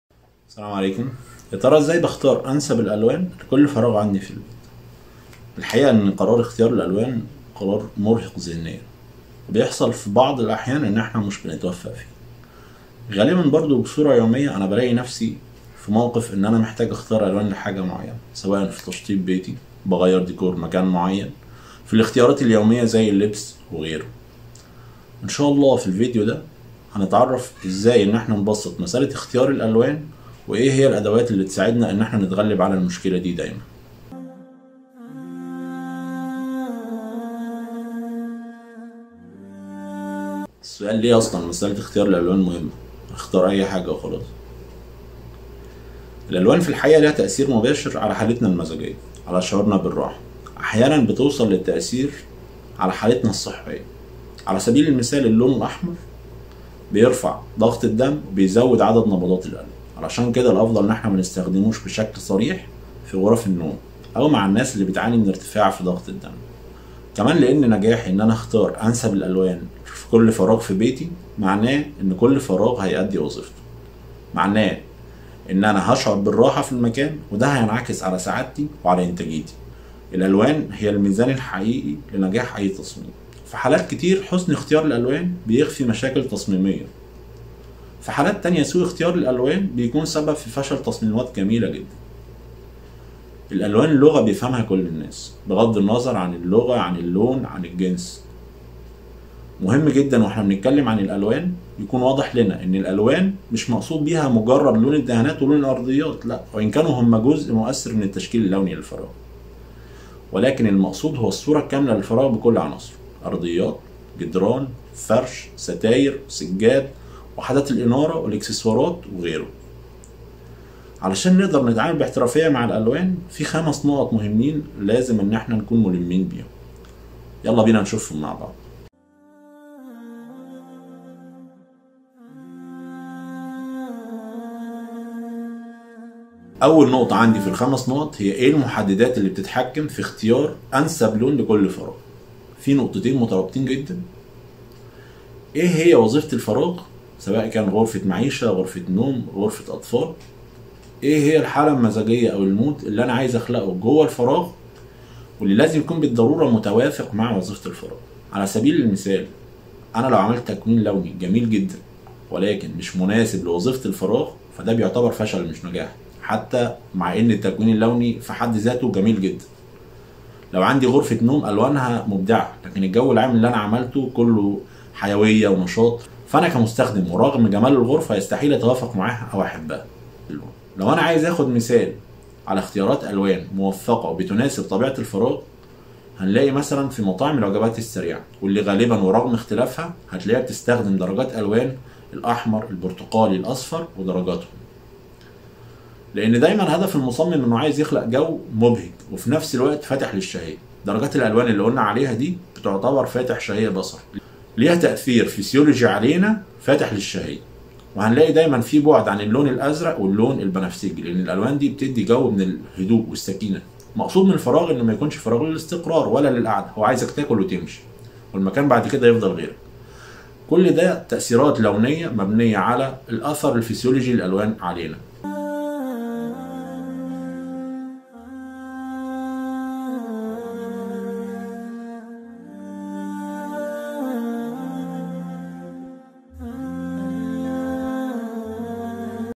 محددات إختيار اللون الأنسب للغرفة ومقدمه عن الألوان-النقطة الأولى-اختيار انسب لون لكل غرفة فى بيتك
محددات إختيار اللون الأنسب للغرفة ومقدمه عن الألوان
الفيديو هو جزء من فيديو شامل عن كيفية إختيار أنسب الألوان لبيتك وكيفية عمل أفضل صيغ التناغم اللونى

وصف الفيديو الكامل فى هذا الفيديو نستعرض شرح مبسط لكيفية اختيار وتنسيق الألوان
وكذلك قواعد وأساسيات اختيار الالوان القابلة للتطبيق فى كل المجالات الحياتية "الديكور ,التصميمات وحتى إختيار الملابس" وخاصة تطبيقات التصميم الداخلى
تنبع أهمية الألوان من تأثيرها المباشر على حالتنا المزاجية وكذلك على شعورنا بالراحة وكذلك حالتنا الصحية
الفيديو ينقسم إلى خمسة محاور أساسية :
1- محددات إختيار أنسب الألوان لكل فراغ.
2- فهم نظرية الألوان.
3- طبيعة وتأثير الألوان .
4- طرق إستخدام دائرة الألوان للحصول على التناغم اللونى الأمثل.
5- نصائح عامة.